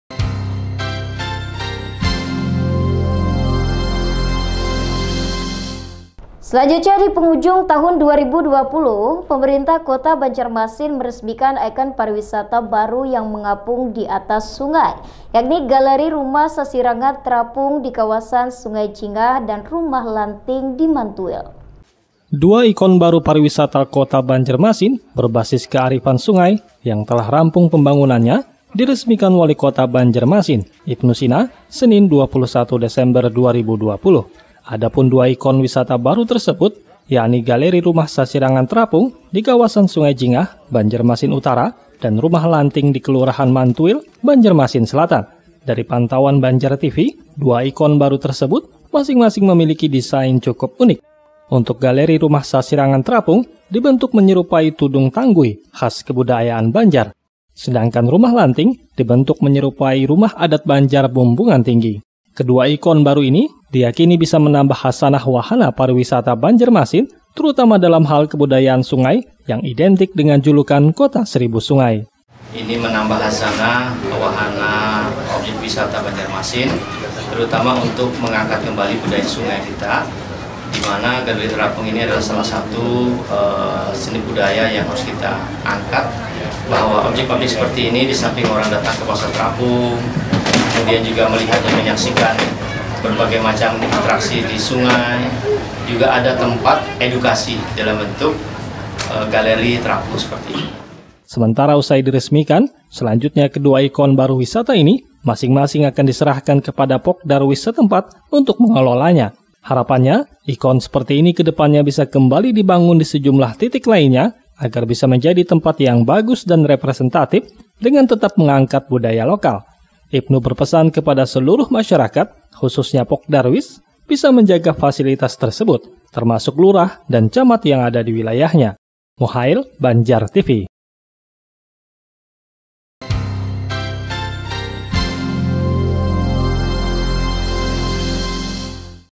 PEMKO BANJARMASIN RESMIKAN 2 IKON PARIWISATA BARU
Dipenghujung tahun 2020, Pemerintah Kota Banjarmasin meresmikan ikon pariwisata baru yang mengapung di atas sungai yakni Galeri Rumah Sasirangan Terapung di kawasan Sungai Jingah dan Rumah Lanting di Mantuil.